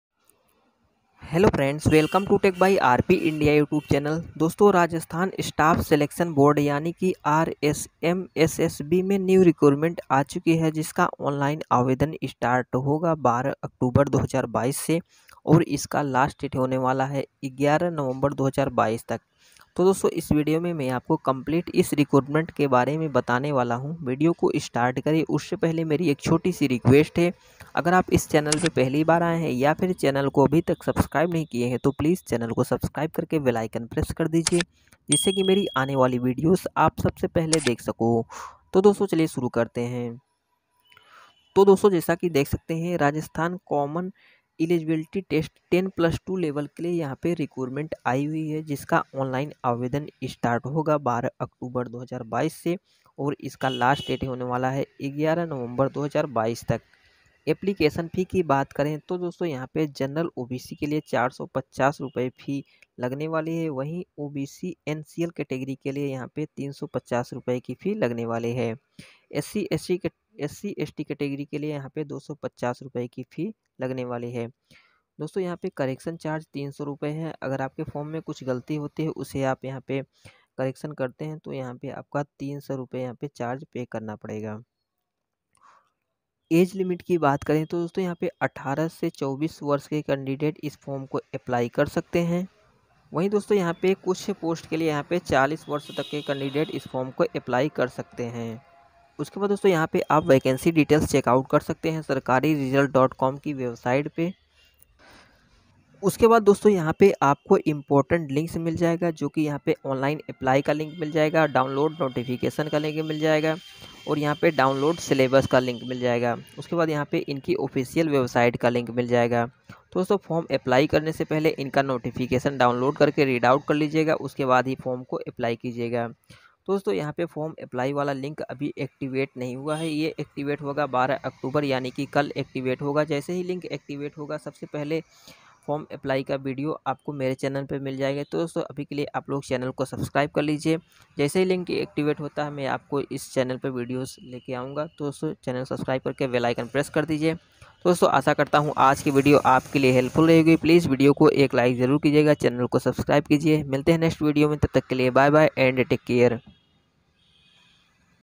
RSMSSB CET 10+2 Level Online Form 2022 Kaise Bhare || How to Fill RSMSSB CET 10+2 Level Form 2022
My mice